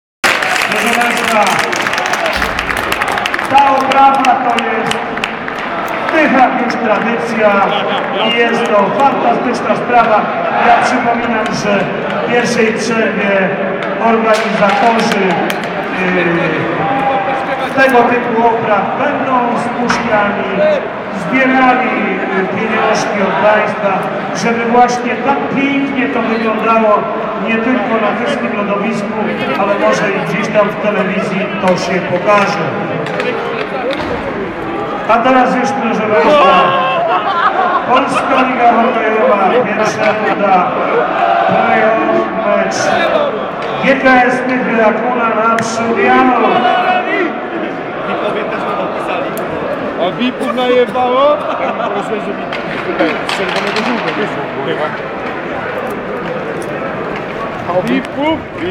GKS Tychy & Naprzod Janow (Oprawa meczu w pierwszej rundzie play-off)
Oprawa meczu w pierwszej rundzie play-off, GKS,ŁKS,Cracovia,Zawisza,Sandecja,Gornik Walbrzych,Ultras,Hokej.